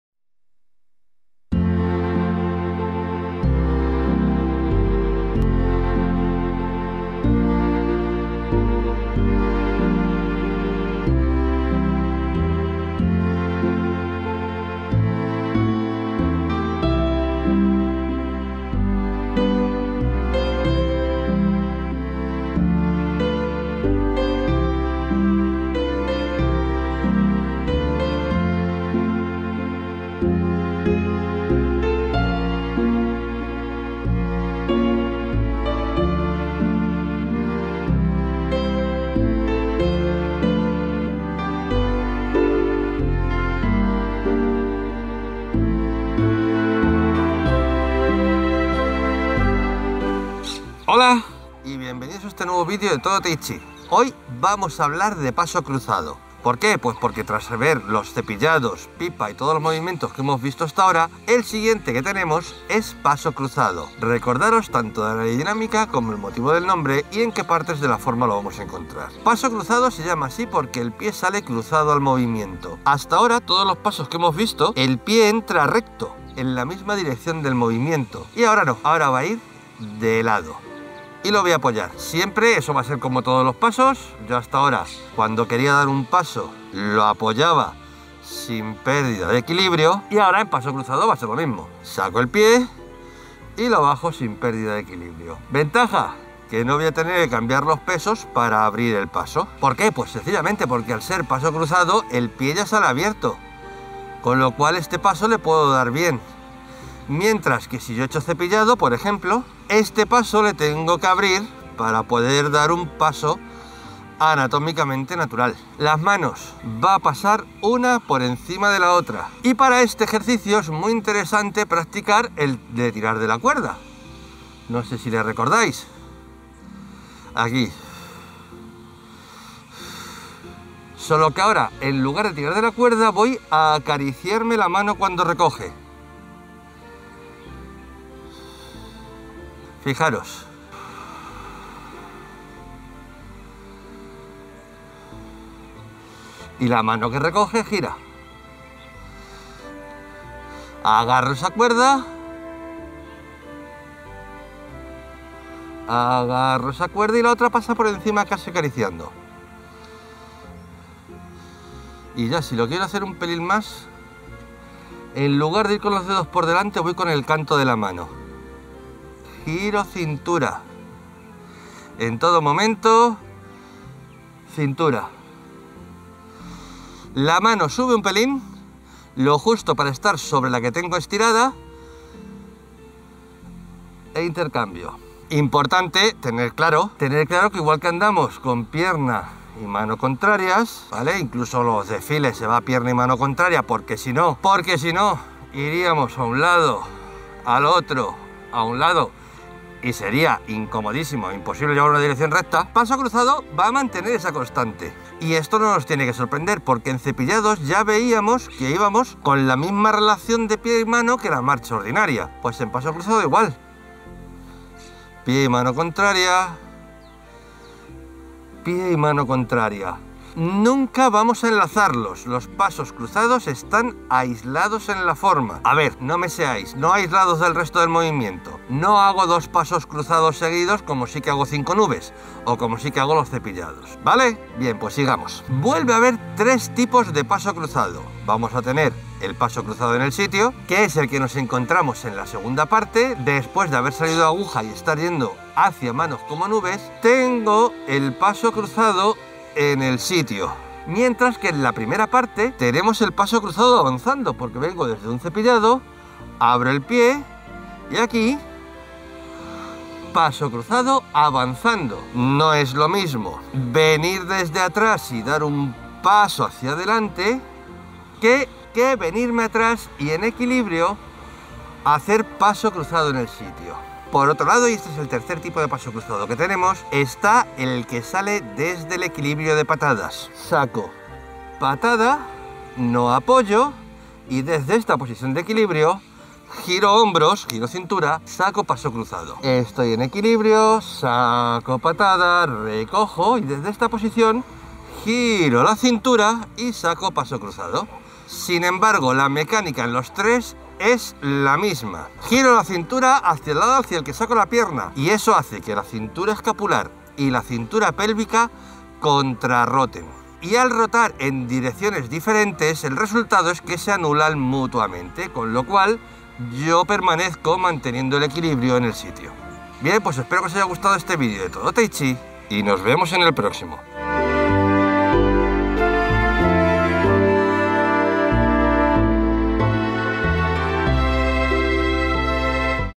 Movimientos Paso Cruzado
Seguimos viendo los movimientos de la forma tradicional del estilo Yang.
Espero que os resulte interesante.